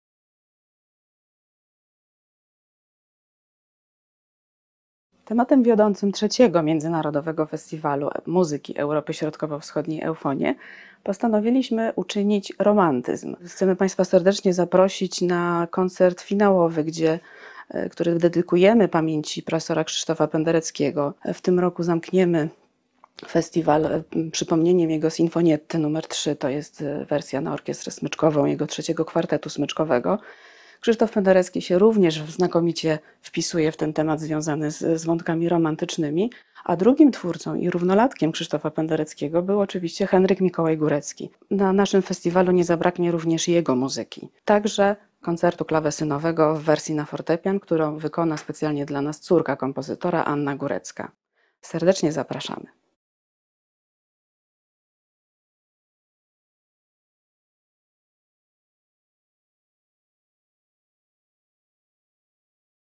Zapowiedź Eufonie 2020 - dr hab. Beata Bolesławska-Lewandowska
Narodowe Centrum Kultury zaprasza na trzecią edycję Festiwalu Eufonie.

Od 20 do 28 listopada w Warszawie. Koncerty na żywo i w streamingu.